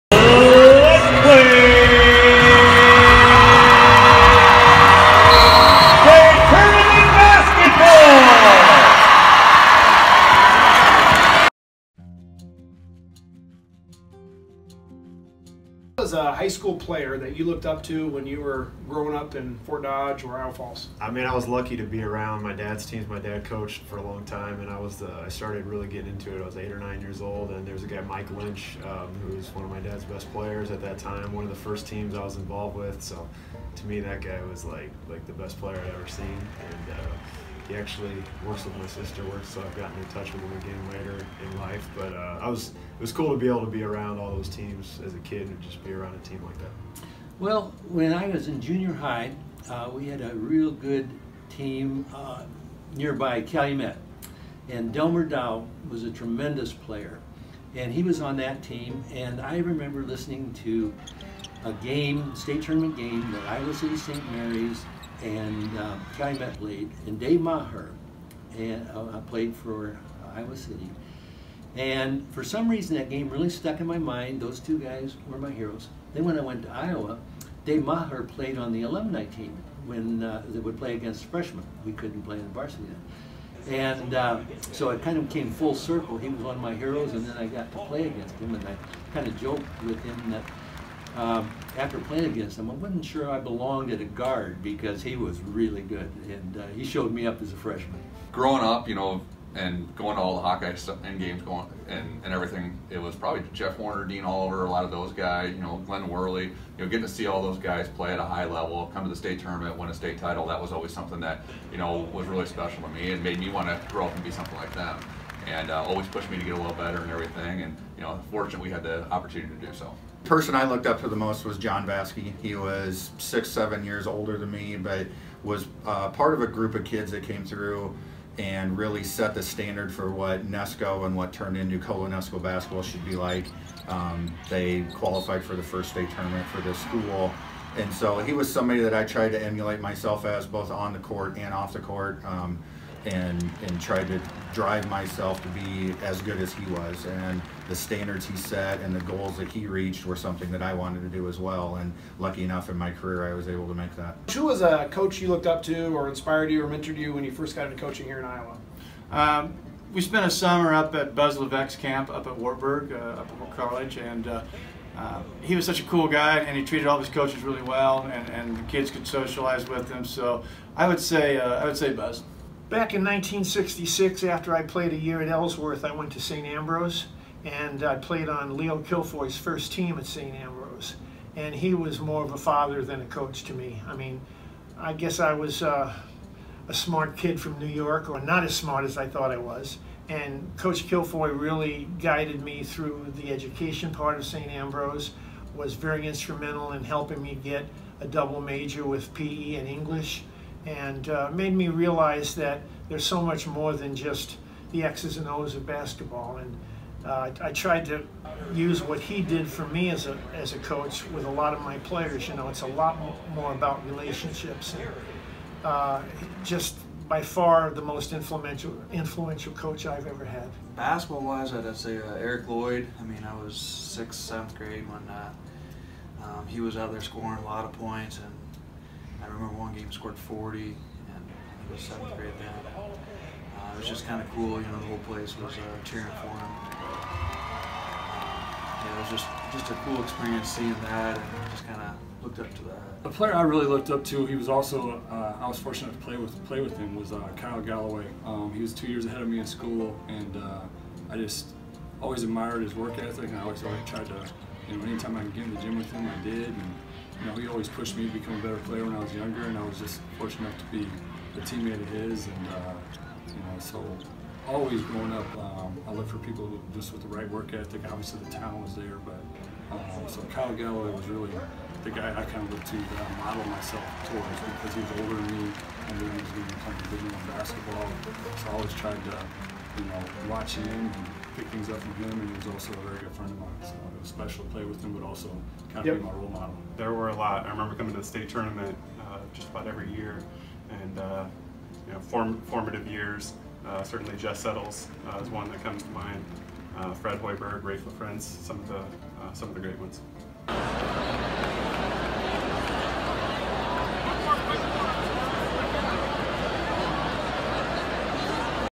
IHSAA 2019 Boys Basketball Hall of Fame Class
Listen to the interviews from the 2019 Hall of Fame Inductees!